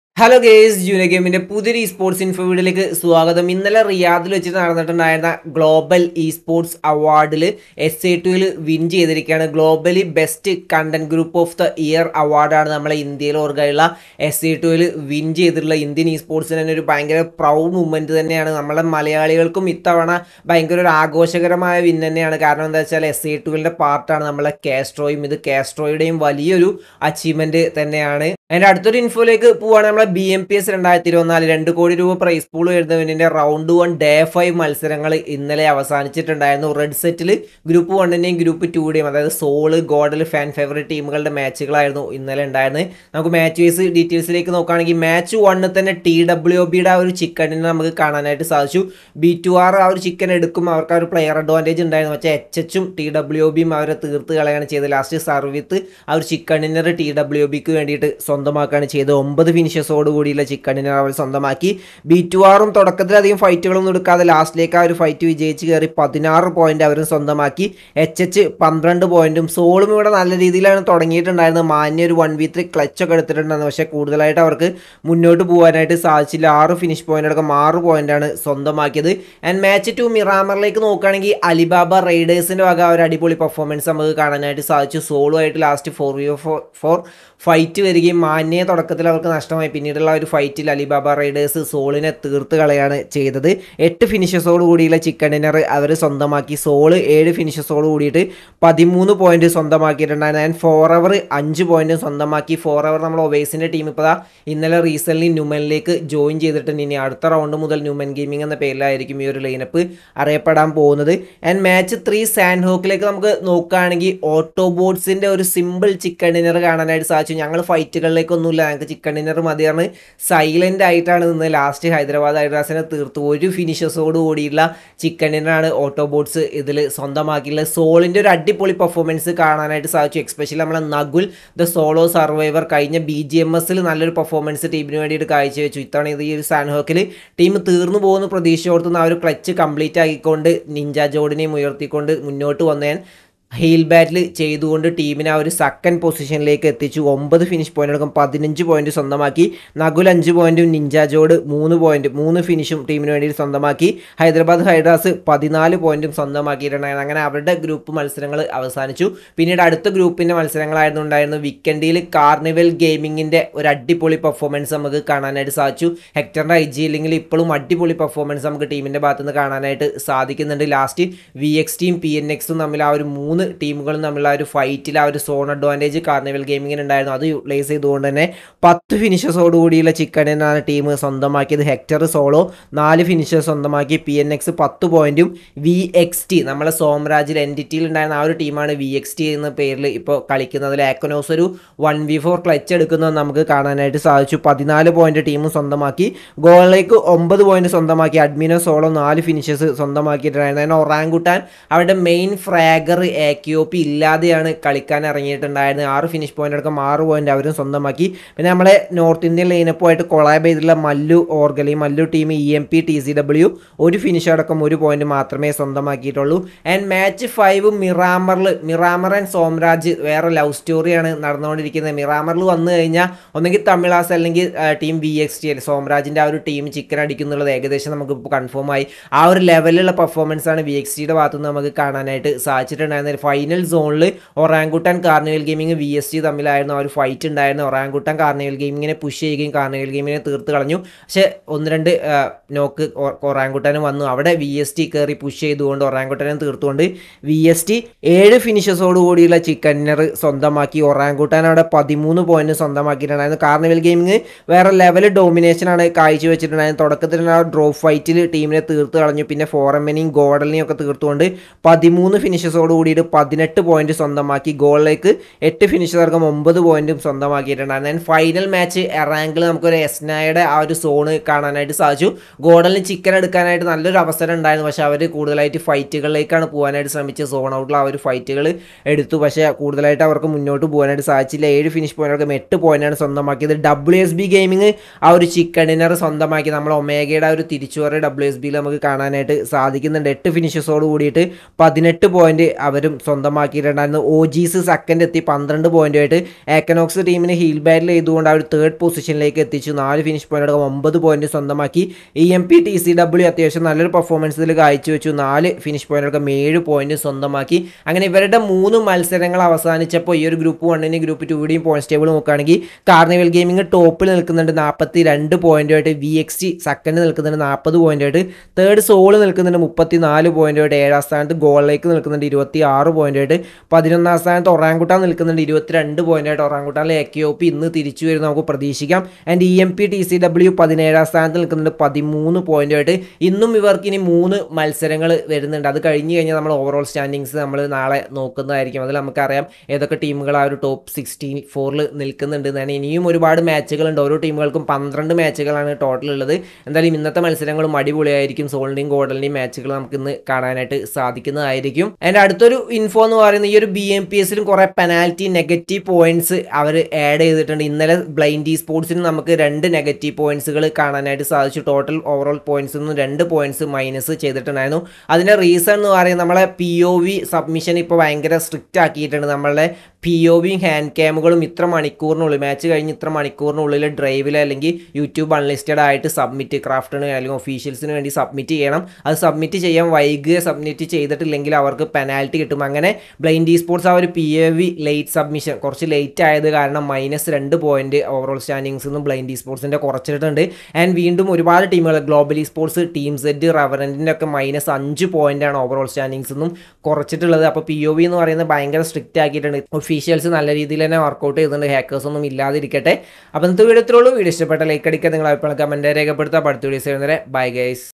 Blind Esports Got Penalty😱Pov issues | Kaztro🚀 S8UL Won Award🏆| CG Domination BMPS | Junior Gaming
Uploading BGMI Esports News | Content Videos & Latest Update

Junior Gaming

Place : Malappuram